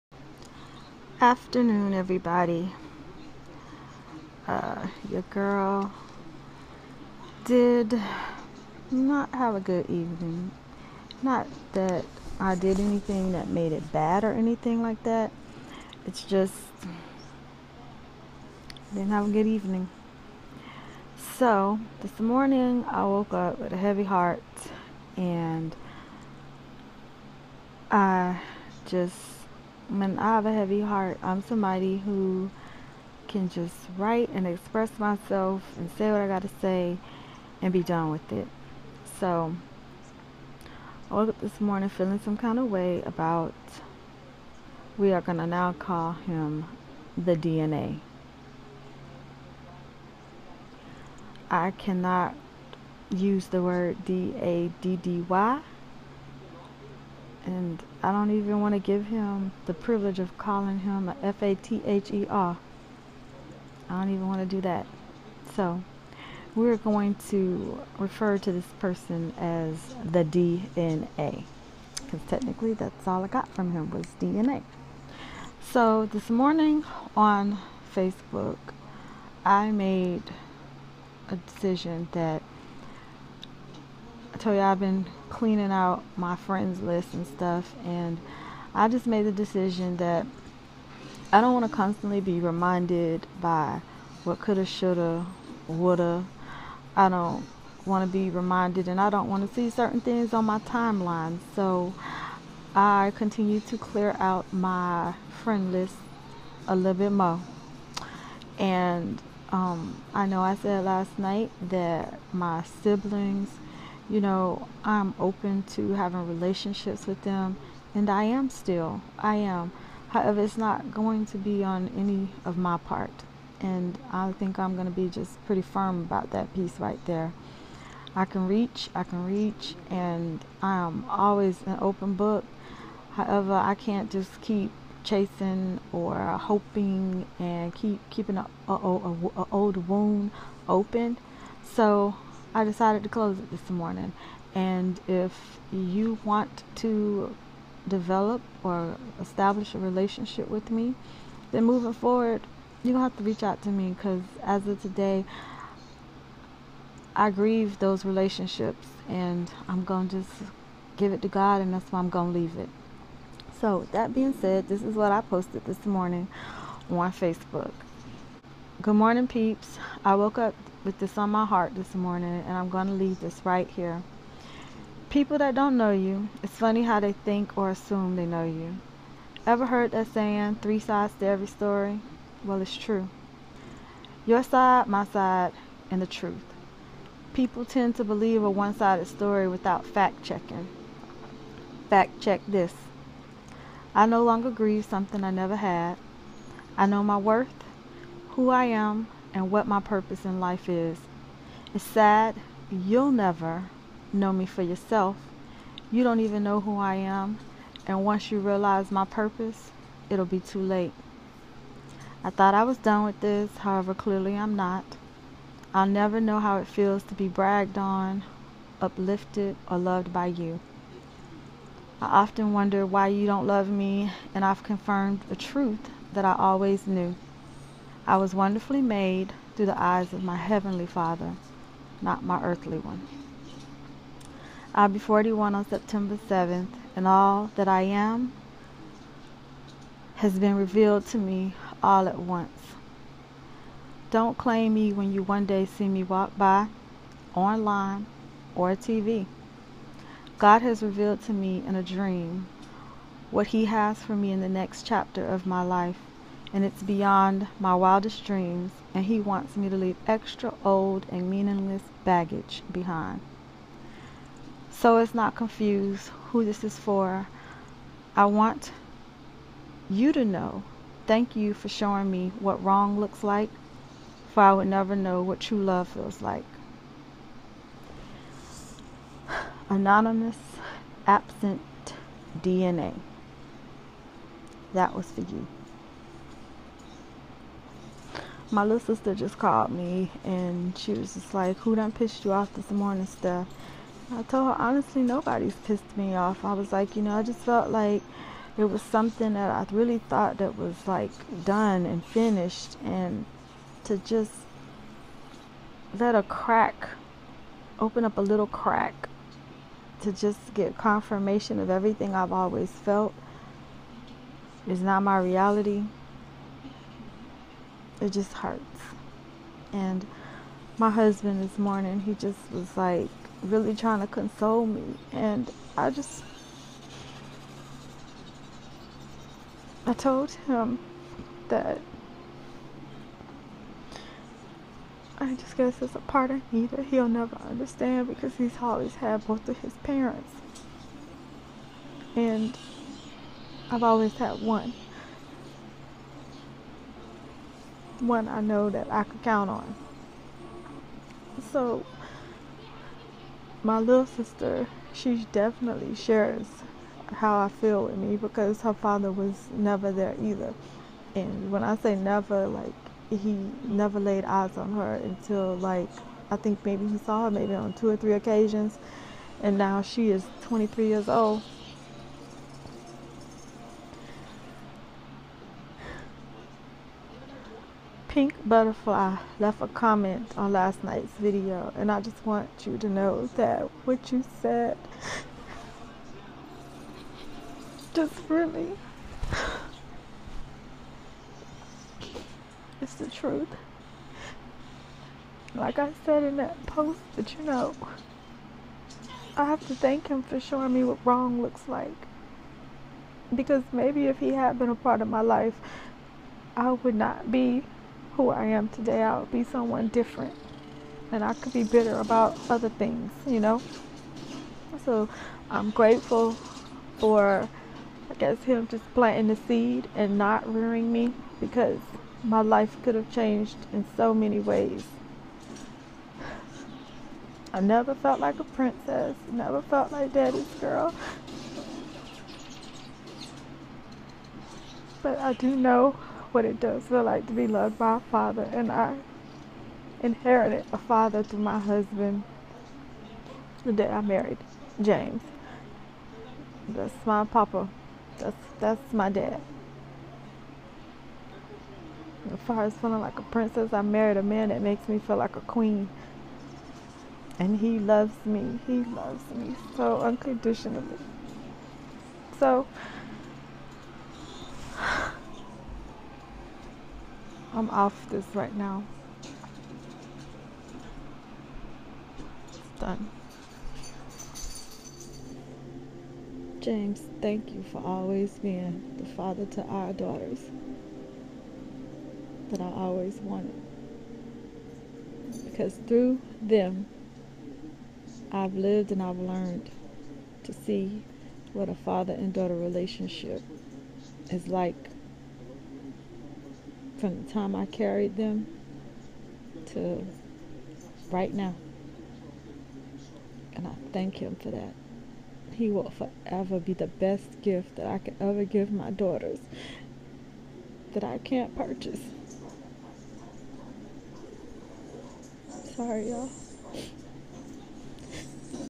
Daily Vlog #41-PT 1- DNA aka Daddy Not Available- REAL TALK!
I'm sorry for this, however I needed to set this out into the universe, so I can be FREE of it!  My Facebook post this morning:

Gm peeps! I woke up with this on my heart this morning and I'm going to leave this right here. People, that don't know you, it's funny how they think/assume they know you. Ever heard that saying, three sides to every story. it's true. Your side, my side and the truth! People tend to believe a one sided story, without fact checking. Fact check this! I no longer GRIEVE something I never had! I know my worth, who I am and what my purpose is in life. Its sad, you'll never know me for yourself, you don't even know who I am and once you realize my purpose...it'll be too late! I thought I was done with this, however clearly I'm not! I'll never know how it feels to be bragged on, uplifted or loved by you. I often wondered why you don't love me and I've confirmed a truth I always knew. I was wonderfully made thru the eyes of my Heavenly FATHER, not my earthly one. I'll be 41 on Sept. 7th and ALL that I am has been revealed to me all at once. Don't claim me when you one day see me walk by, online or TV! God has revealed to me in a dream, what He has for me in this next chapter of my life is beyond my dreams and He wants me to leave extra, old and meaningless baggage behind. So it's not confused, who this is for, I want you to know thank you for showing me what wrong looks like, for I would never know what true love feels like. Anonymous absent DNA

P.S. James I love you, for loving me and always having my back! _________________________________________________________________________________________them!